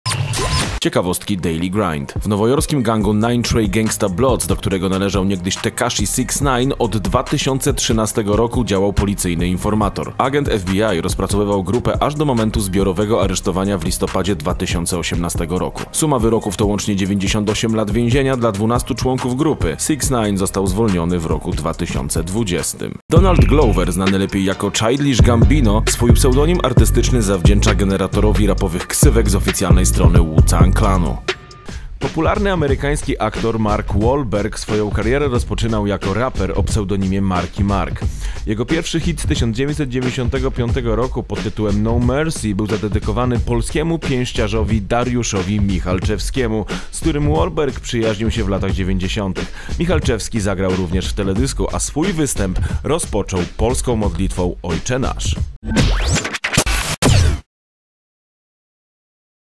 RAPOWE CIEKAWOSTKI ODC.1 / SixNine, Childish Gambino, Dariusz Michalczewski
Zapraszamy do sprawdzenia naszego nowego formatu, w którym przedstawiamy Wam 3 losowe ciekawostki ze świata rapu!

Piszcie w komentarzach o jakich raperach chcielibyście kolejne odcinki!

Czyta: @mcrobak
Muzyka: @prodbybodhi